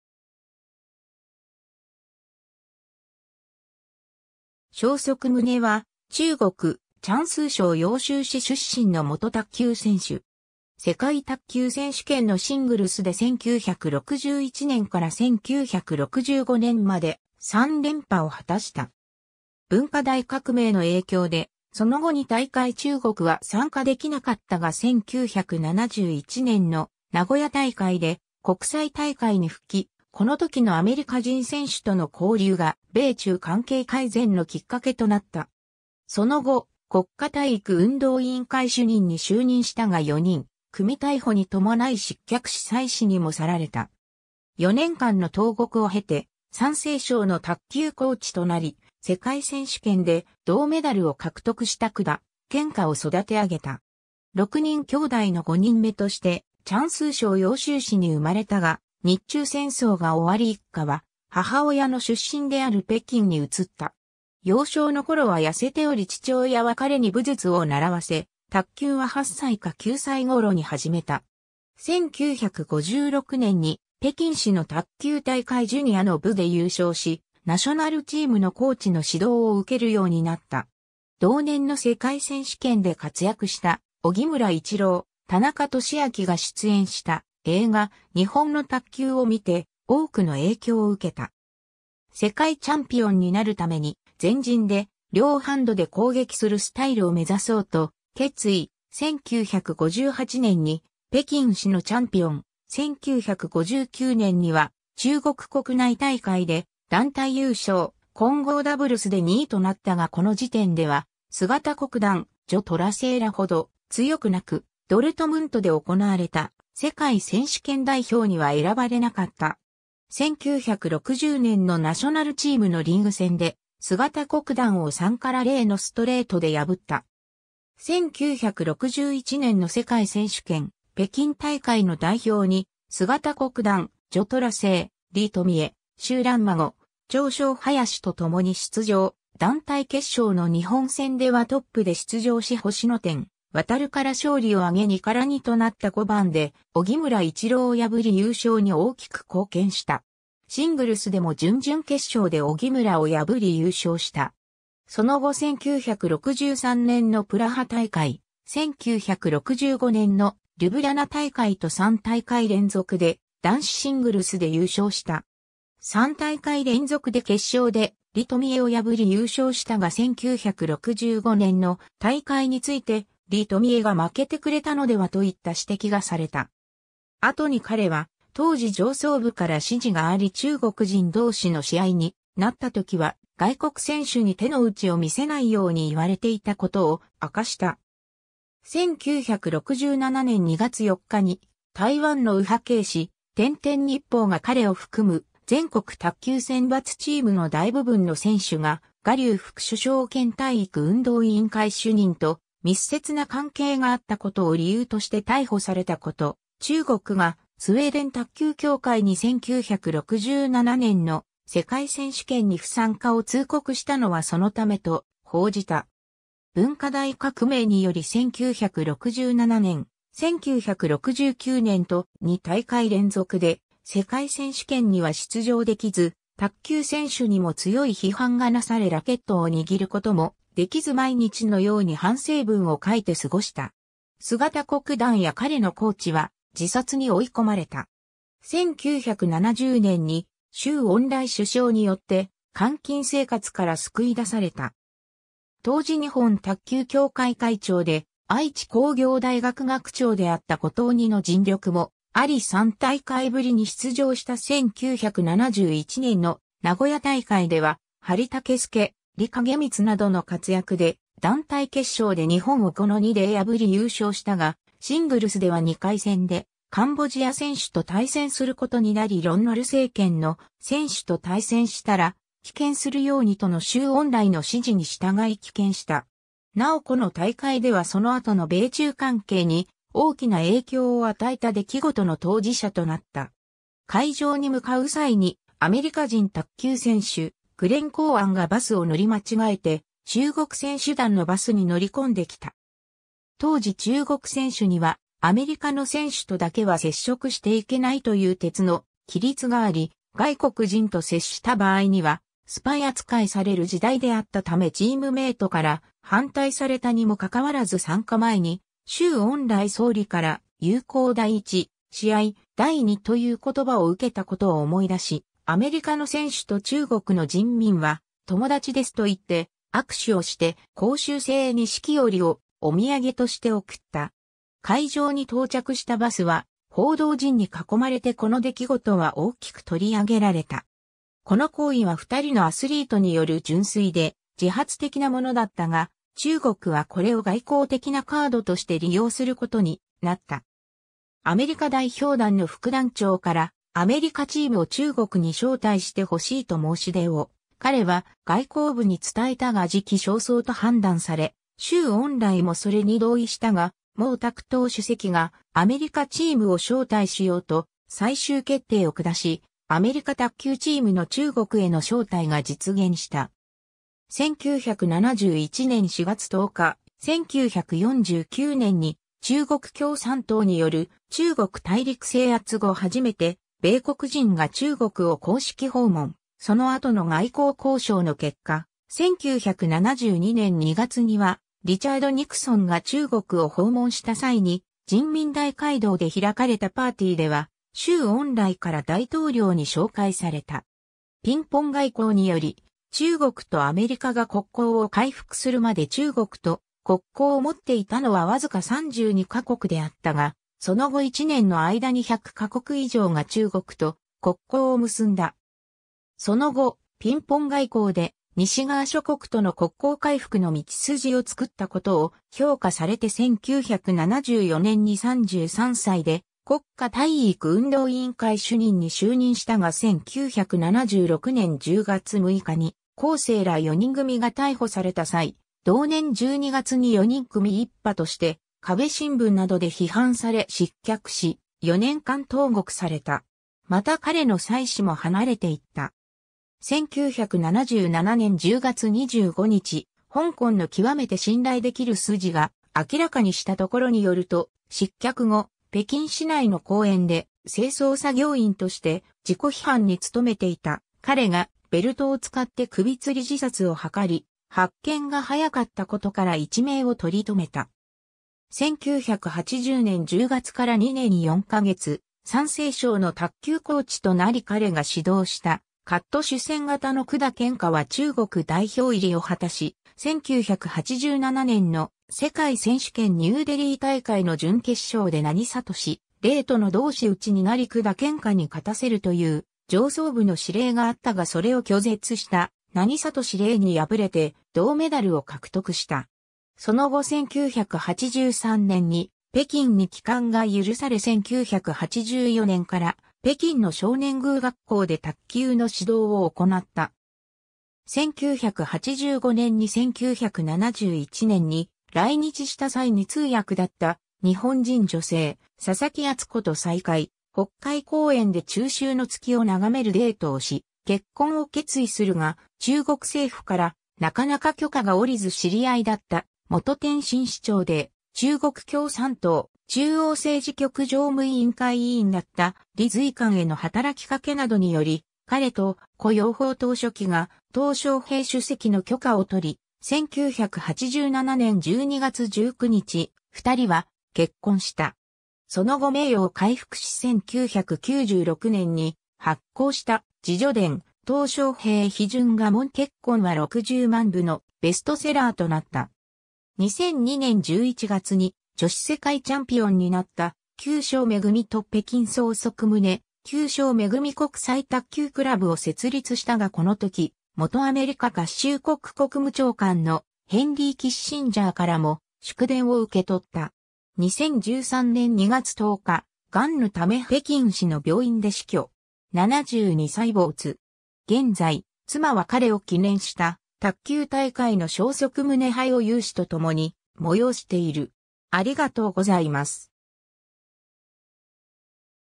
荘則棟
荘則棟（そうそくとう、ツァンヅートン、Zhuang Zedong、1940年8月7日 - 2013年2月10日）は、中国・江蘇省揚州市出身の元卓球選手。
世界卓球選手権のシングルスで1961年から1965年まで3連覇を果たした。
文化大革命の影響でその後2大会中国は参加できなかったが1971年の名古屋大会で国際大会に復帰、この時のアメリカ人選手との交流が米中関係改善のきっかけとなった。
その後、国家体育運動委員会主任（スポーツ大臣）に就任したが四人組逮捕に伴い失脚し妻子にも去られた。
4年間の投獄を経て山西省の卓球コーチとなり世界選手権で銅メダルを獲得した管建華を育て上げた。
 6人兄弟の5人目として江蘇省揚州市に生まれたが日中戦争が終わり一家は母親の出身である北京に移った。
幼少の頃はやせており父親は彼に武術を習わせ、卓球は8歳か9歳頃に始めた。
1956年に北京市の卓球大会ジュニアの部で優勝しナショナルチームのコーチの指導を受けるようになった。
同年の世界選手権で活躍した荻村伊智朗、田中利明が出演した映画「日本の卓球」を見て多くの影響を受けた。
世界チャンピオンになるために前陣で両ハンドで攻撃するスタイルを目指そうと決意、1958年に北京市のチャンピオン、1959年には中国国内大会で団体優勝、混合ダブルスで2位となったがこの時点では容国団、徐寅生らほど強くなくドルトムントで行われた世界選手権代表には選ばれなかった。
 1960年のナショナルチームのリーグ戦で容国団を3-0のストレートで破った。
1961年の世界選手権北京大会の代表に容国団、徐寅生、李富栄、周蘭孫、張燮林とともに出場、団体決勝の日本戦ではトップで出場し星野展弥から勝利をあげ2-2となった5番で荻村伊智朗を破り優勝に大きく貢献した。
シングルスでも準々決勝で荻村を破り優勝した。
その後1963年のプラハ大会、1965年のリュブリャナ大会と3大会連続で男子シングルスで優勝した。
3大会連続で決勝で李富栄を破り優勝したが1965年の大会について李富栄が負けてくれたのではといった指摘がされた。
後に彼は当時上層部から指示があり中国人同士の試合になったときは外国選手に手の内を見せないように言われていたことを明かした。
 1967年2月4日に、台湾の右派系紙、天々日報が彼を含む全国卓球選抜チームの大部分の選手が賀竜副首相兼体育運動委員会主任と密接な関係があったことを理由として逮捕されたこと、中国がスウェーデン卓球協会に1967年の世界選手権に不参加を通告したのはそのためと報じた。
 文化大革命により1967年、1969年と2大会連続で世界選手権には出場できず卓球選手にも強い批判がなされラケットを握ることもできず毎日のように反省文を書いて過ごした。
容国団や彼のコーチは自殺に追い込まれた。
 1970年に周恩来首相によって監禁生活から救い出された。
当時日本卓球協会会長で愛知工業大学学長であった後藤鉀二の尽力もあり3大会ぶりに出場した1971年の名古屋大会では梁丈亮、李景光などの活躍で団体決勝で日本を5-2で破り優勝したがシングルスでは2回戦でカンボジア選手と対戦することになりロン・ノル政権の選手と対戦したら棄権するようにとの周恩来の指示に従い棄権した。
 なおこの大会ではその後の米中関係に大きな影響を与えた出来事の当事者となった。
会場に向かう際にアメリカ人卓球選手、グレン・コーワンがバスを乗り間違えて中国選手団のバスに乗りこんできた。
当時中国選手にはアメリカの選手とだけは接触していけないという鉄の規律があり、外国人と接した場合にはスパイ扱いされる時代であったためチームメートから反対されたにもかかわらず参加前に周恩来総理から「友好第一、試合第二」という言葉を受けたことを思い出し「アメリカの選手と中国の人民は友だちです」と言って握手をして杭州製錦織（西湖の風景が描かれていた）をお土産として贈った。
会場に到着したバスは報道陣に囲まれてこの出来事は大きく取り上げられた。
この行為は2人のアスリートによる純粋で自発的なものだったが、中国はこれを外交的なカードとして利用することになった。
アメリカ代表団の副団長からアメリカチームを中国に招待してほしいと申し出を彼は外交部に伝えたが時期尚早と判断され、周恩来もそれに同意したが毛沢東主席がアメリカチームを招待しようと最終決定を下...